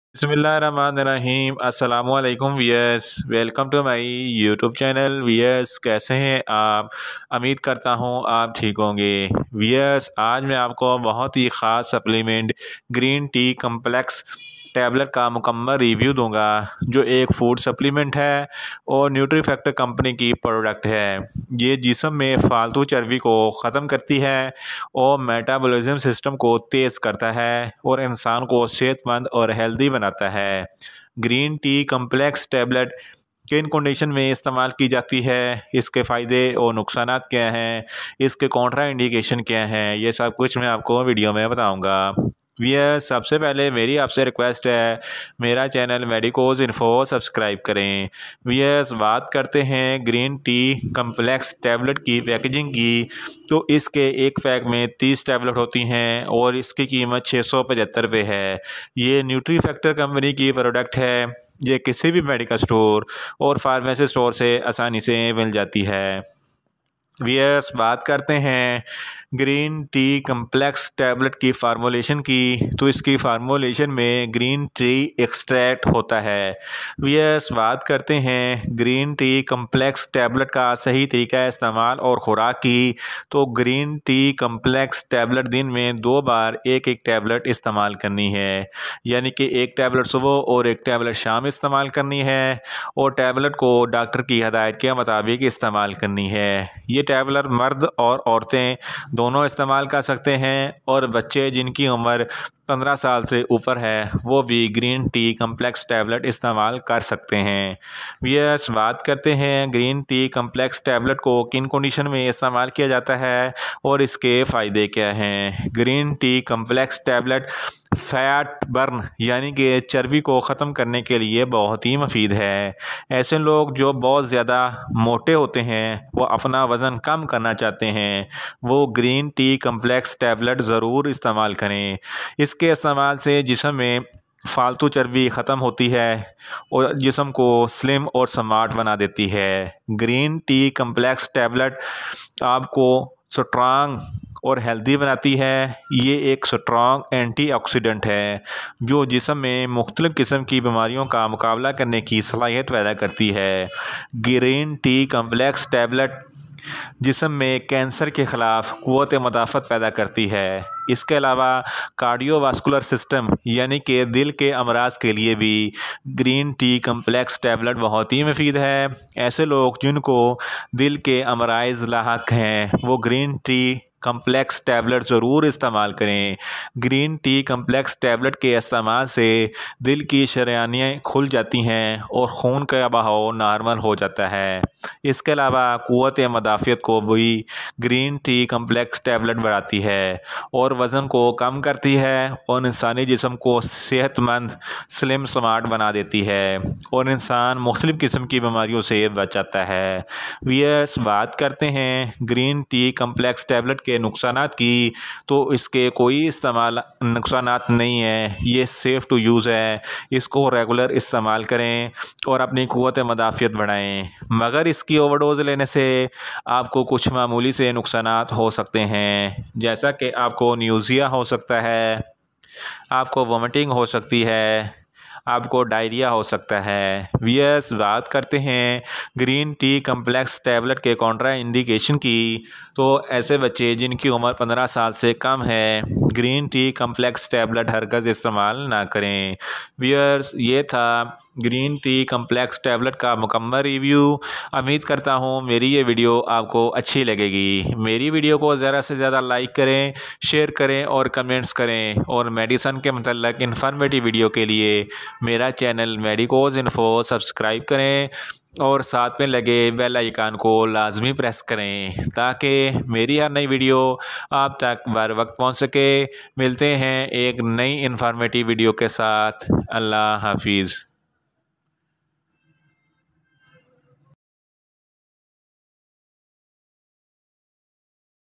How to Lose weight Fast in 7 Days | Green Tea Capsule for weight loss |Nutrifactor Green Tea complex
Green Tea complex tablet uses in weight Fast in 7 Days. Green Tea complex tablet uses, benefits, side effects, dosage and composition Greentea Extact in urdu